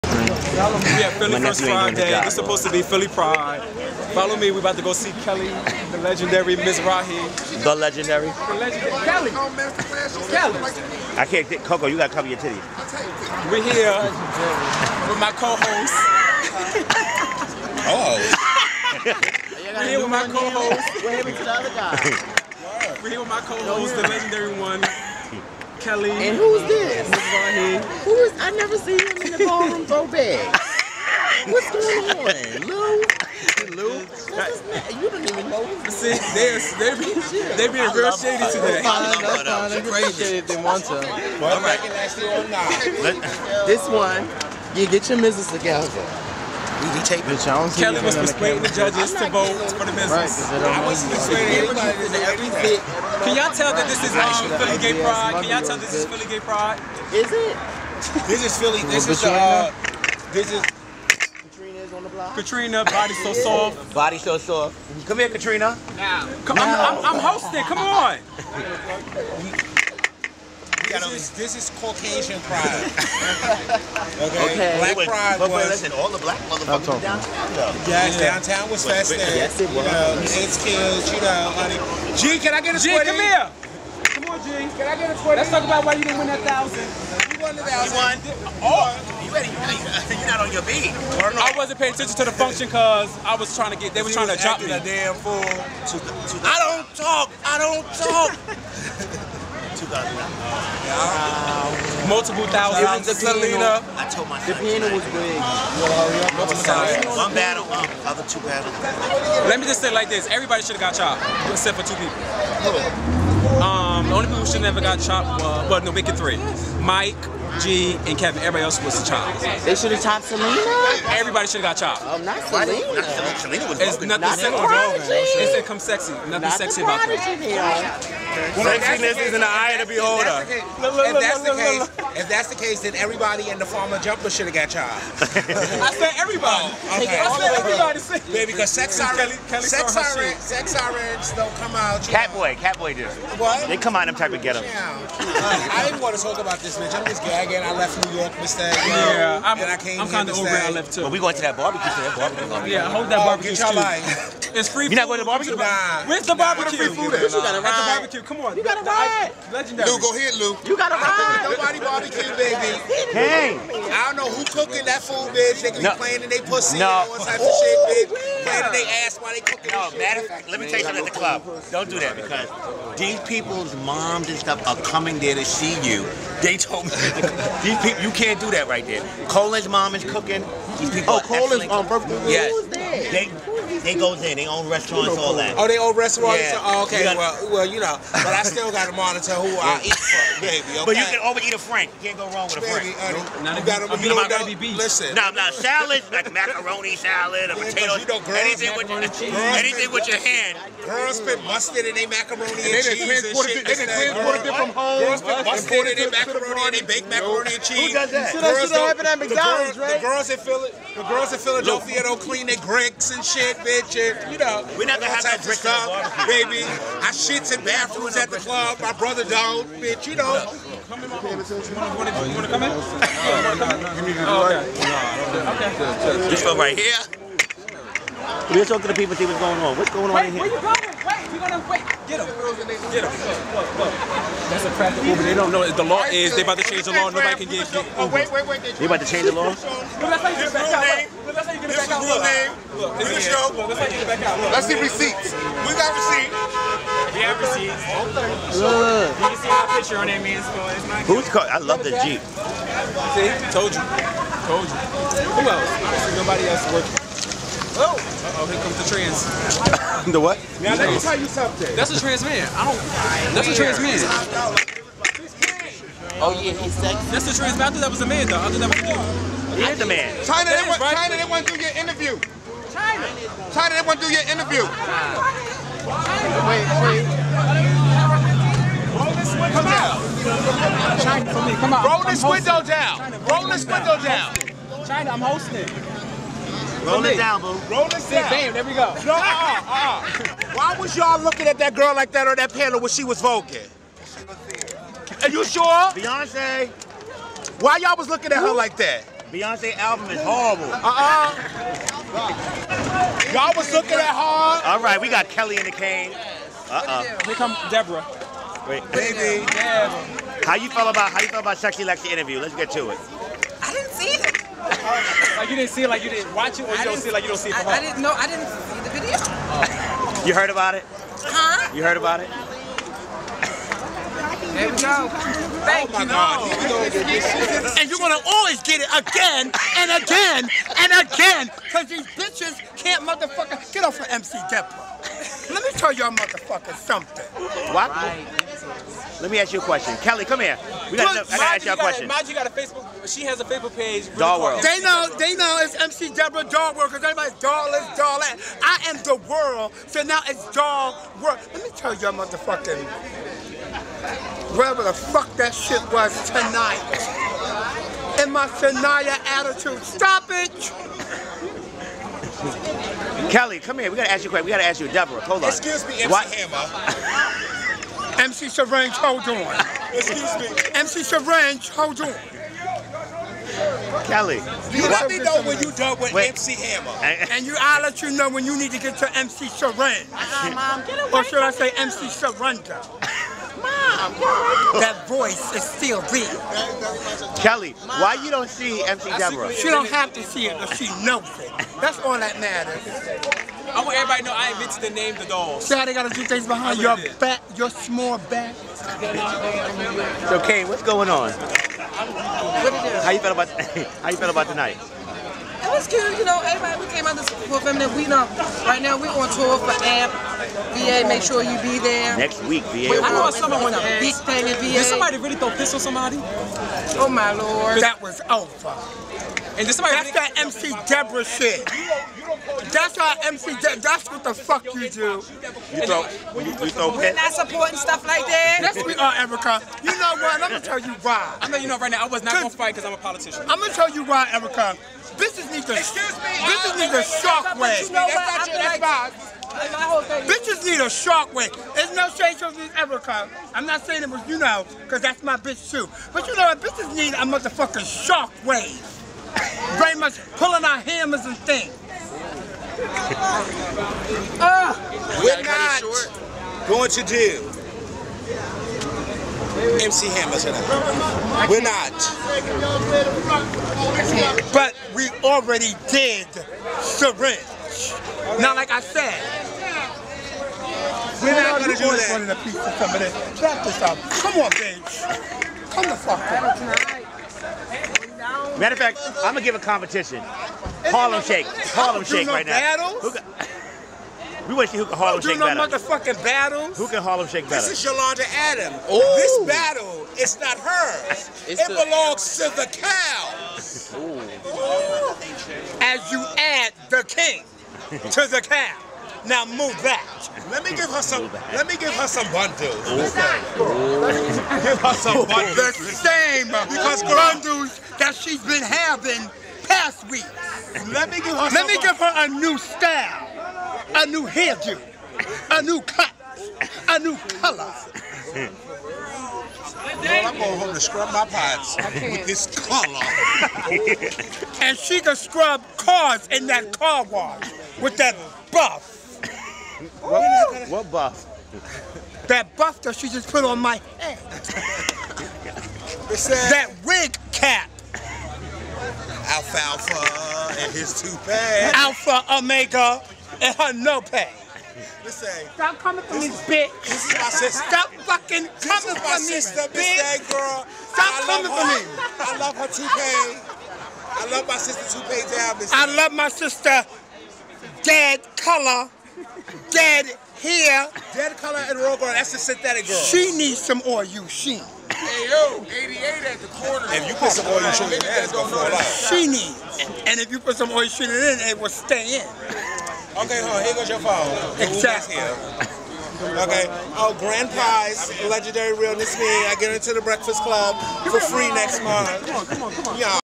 AFTERMATH @ FEMININE DESTRUCTION FIRST FRIDAY 6/6/14 PART 2
AFTERMATH @ FEMININE DESTRUCTION FIRST FRIDAY 6/6/14 PART 2 Check out The Youtube sensations Kelly Mizrahi and McDebra. excerpts from this video has gone viral on social media.